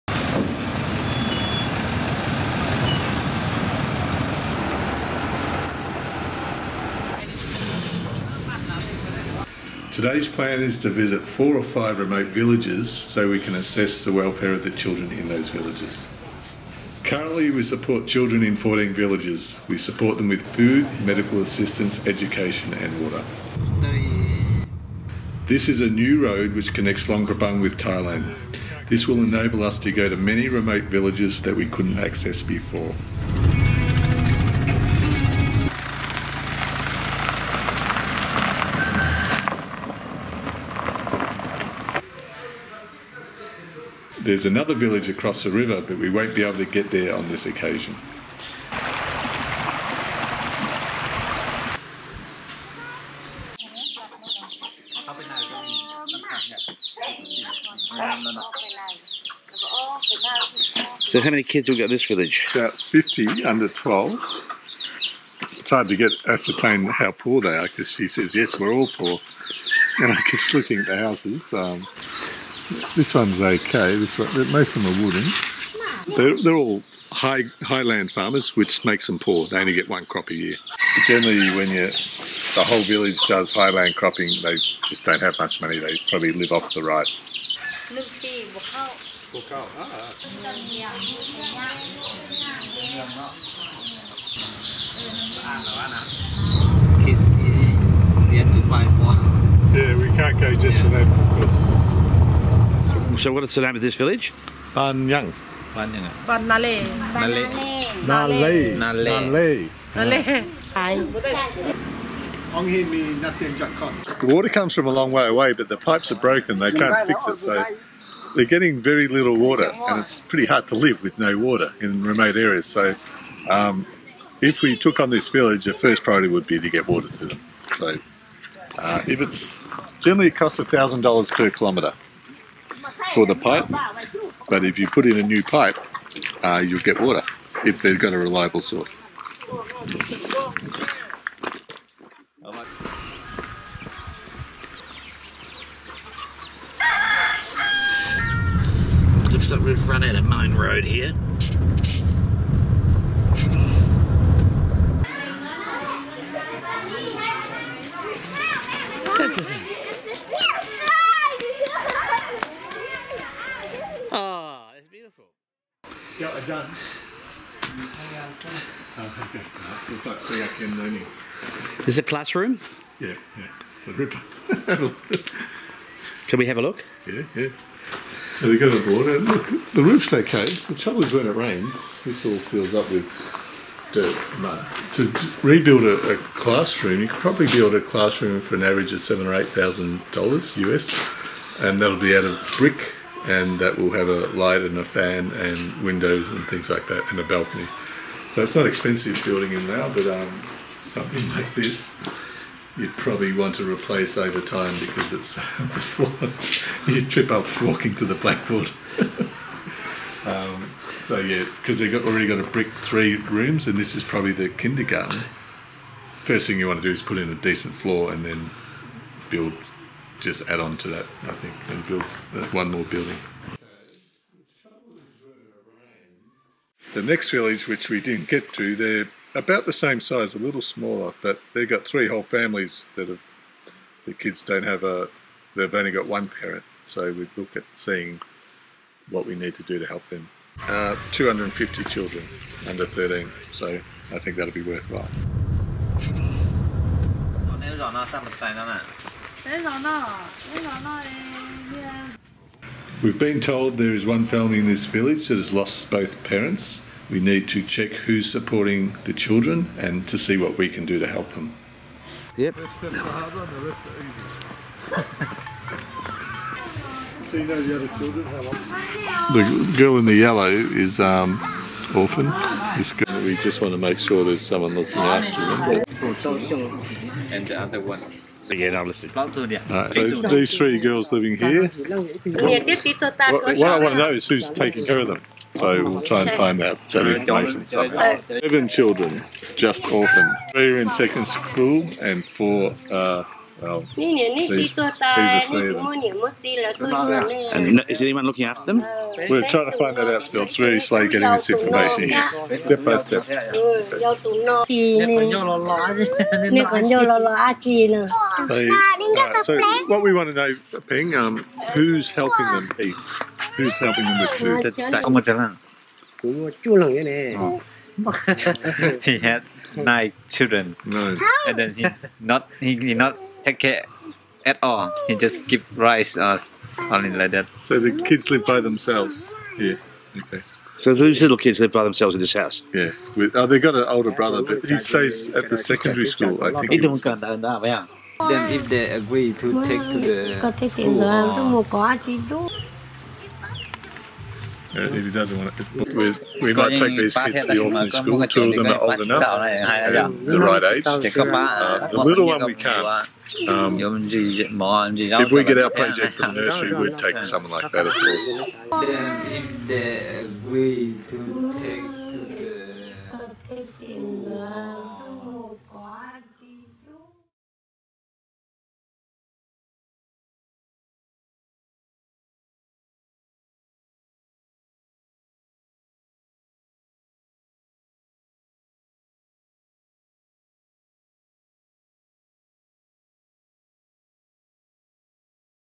Lao Village Support
Support Lao Children assists children in villages and schools around Luang Prabang.
Our mission is to protect and support all young children in Northern Laos with nutritious food,
educational and medical support and clean water.